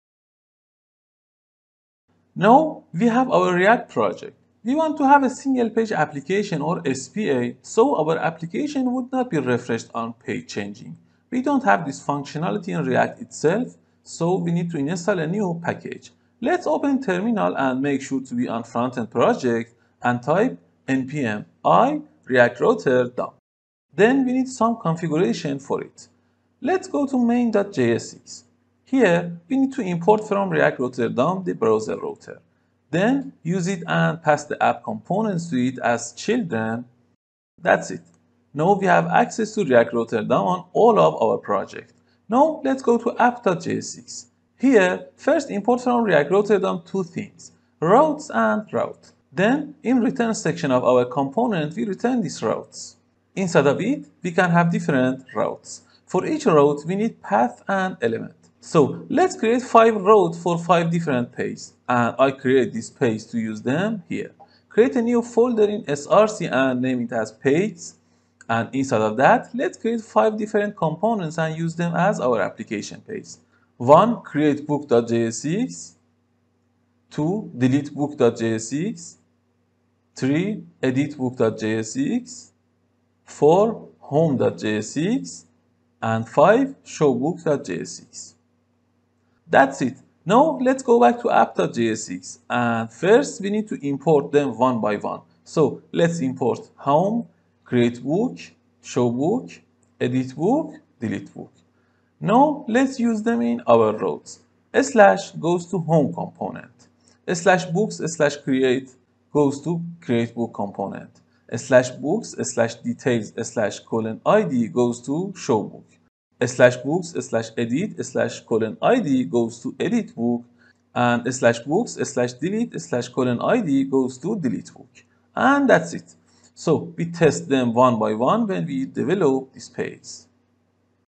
SPA and Add react router dom | Book Store MERN Stack project Lesson 13
Let’s create a MERN stack (MongoDB, Express, React, Node.js) project and learn a lot of topics together.

If you don’t know what is MERN stack, I just released a fast overview of MERN.

We are going to create a basic Book store project. We use MERN stack so we will have
MongoDB
Express js
React
And node js